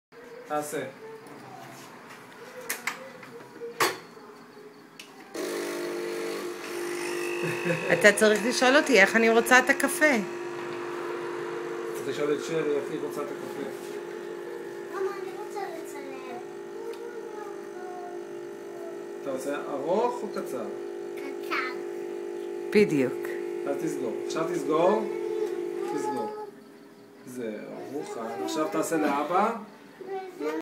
Mini Barista
The best espresso made by a talented cute barista.